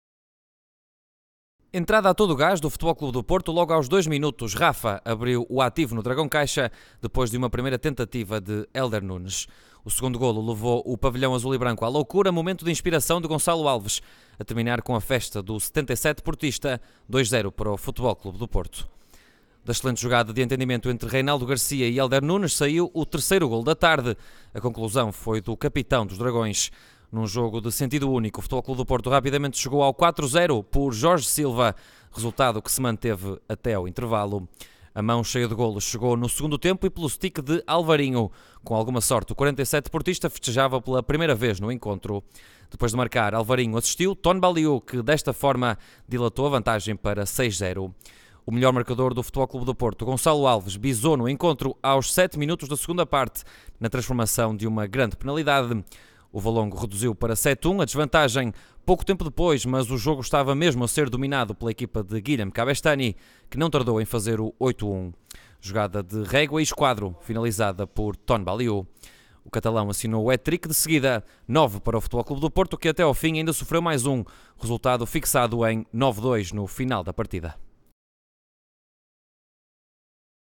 Hóquei em Patins: FC Porto-Valongo, 9-2 (CN, 19.ªj, 28/04/18)
Continua limpo o registo do FC Porto Fidelidade no Campeonato Nacional em 2018: a 11.ª vitória consecutiva, que lhe permite manter-se a um ponto do primeiro lugar da tabela, foi conquistada este sábado, no Dragão Caixa, na receção ao vizinho Valongo, atual quinto classificado, e com números expressivos: 9-2. Ton Baliu assinou um hat-trick e liderou a lista de marcadores de um jogo também assinada por Gonçalo Alves (2), Rafa, Hélder Nunes, Alvarinho e Jorge Silva.